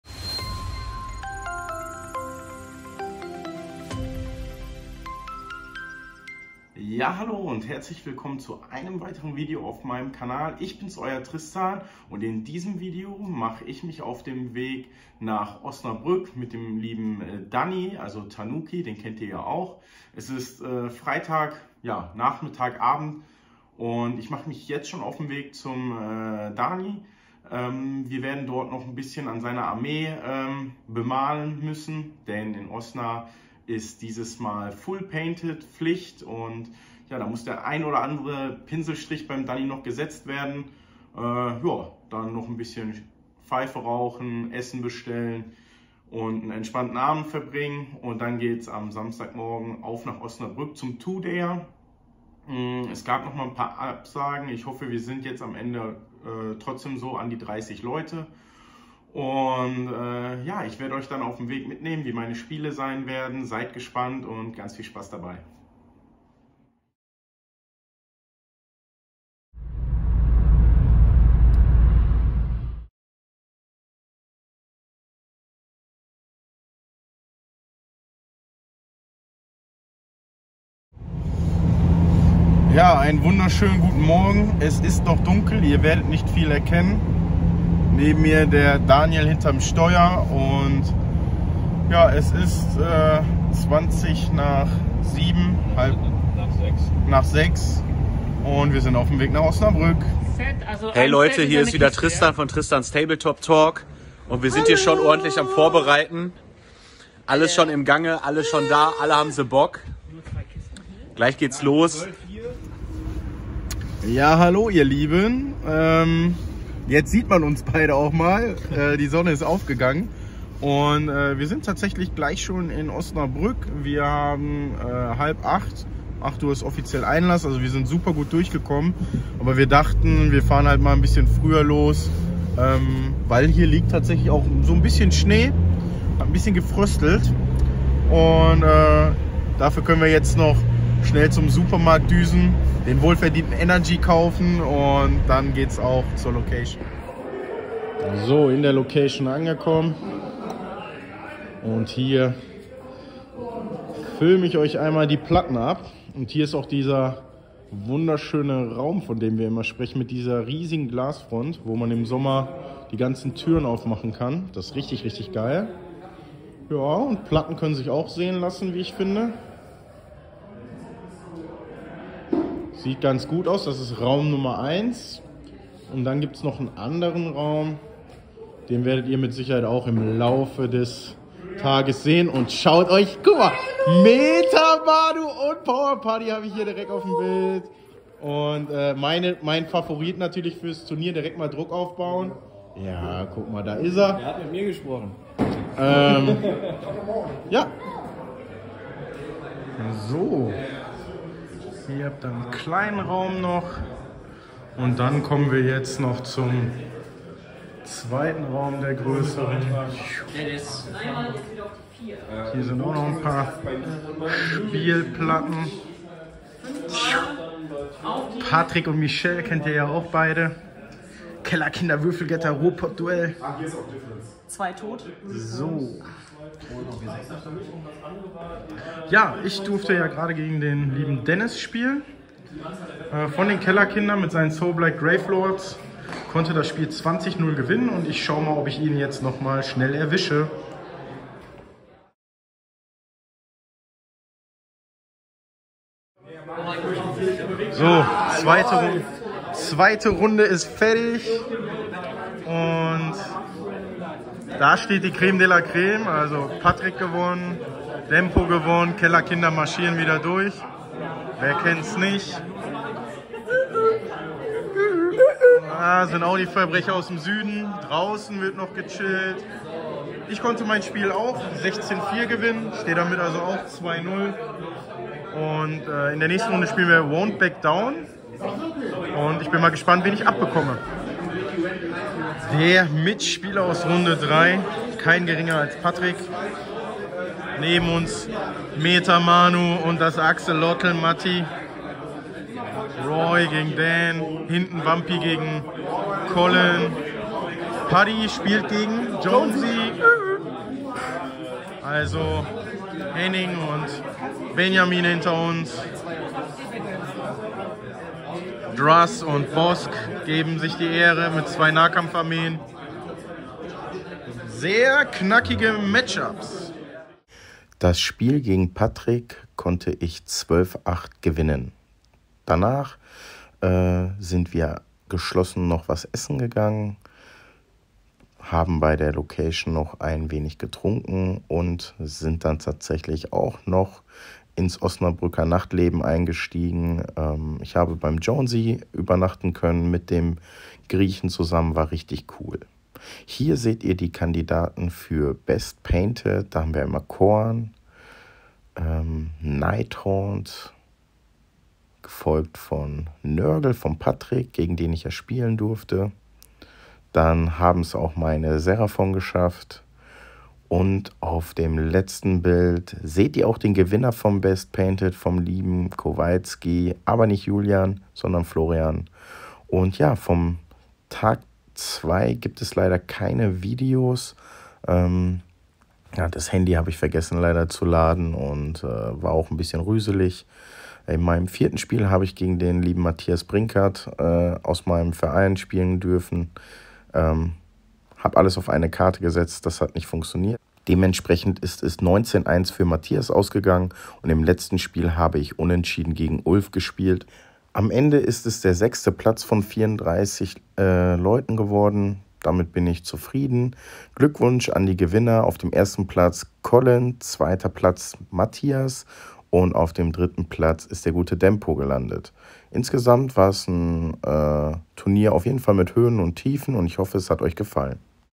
Age of Sigmar 3.1 | Turnierrückblick #7
Ein weiterer 2Dayer in Osnabrück. Kurz und knapp bekommt ihr ein paar Eindrücke wie es war. Leider fehlen Aufnahmen von Tag 2, ich wollte euch dennoch dran teilhaben lassen.